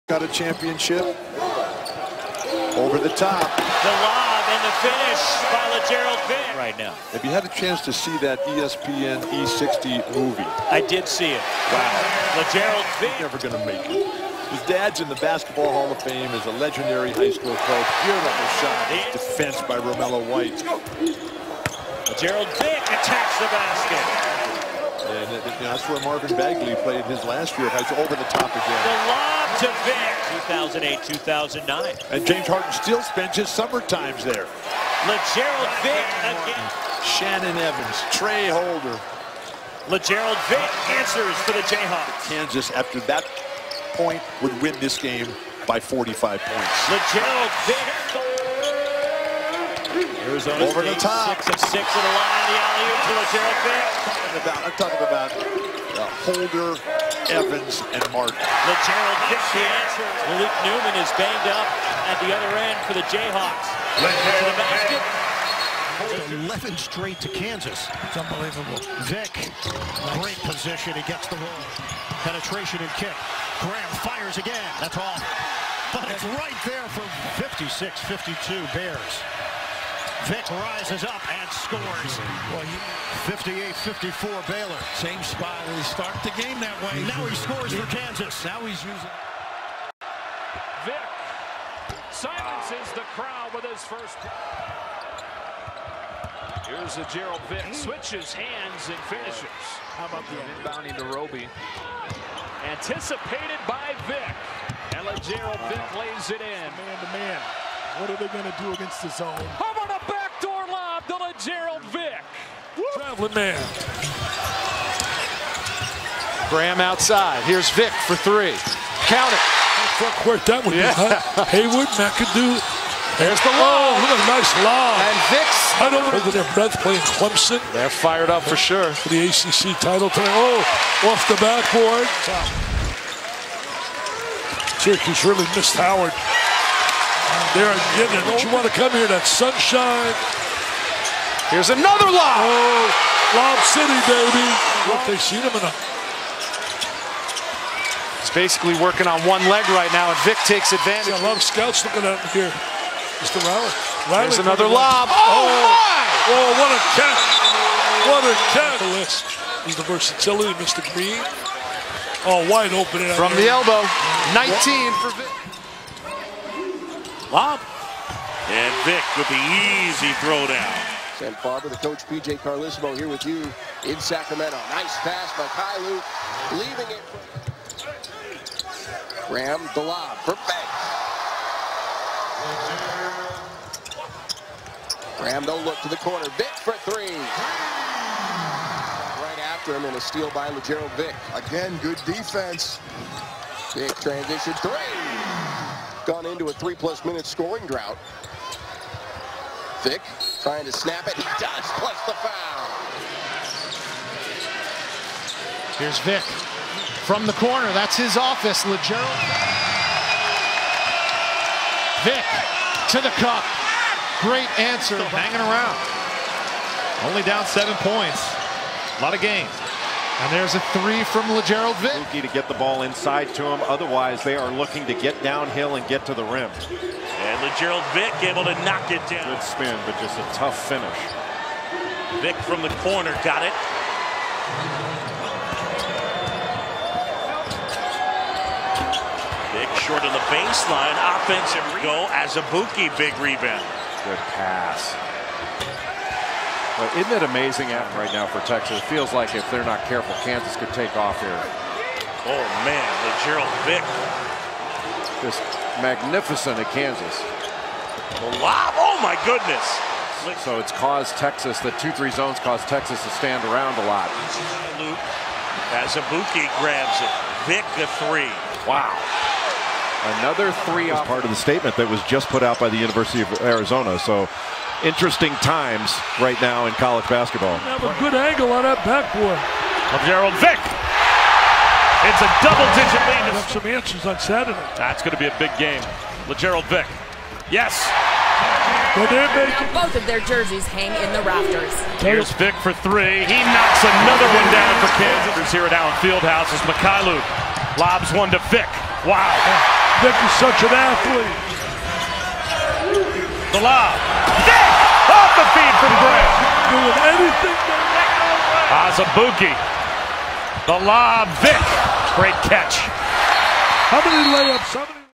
Lagerald Vick 2017-2018 Season Highlights ᴴᴰ | Kansas | 12.1 PPG, 4.8 RPG, 2.1 APG
Highlights from Lagerald Vick's junior season with Kansas.